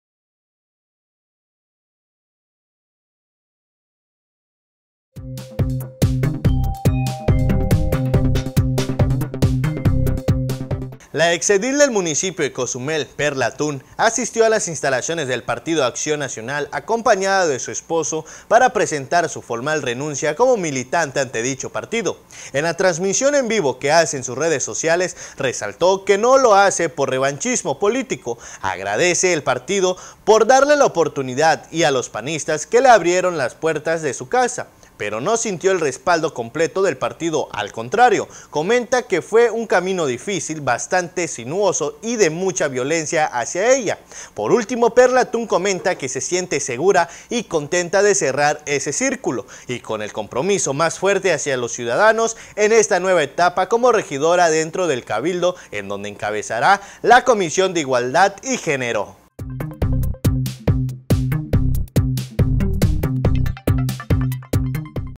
PERLA TUN GENIO Y FIGURA EN COZUMEL, RENUNCIA AL PAN POR FALTA DE APOYO
Entérate de las noticias “EN CORTO”…PERLA TUN GENIO Y FIGURA EN COZUMEL, RENUNCIA AL PAN POR FALTA DE APOYO...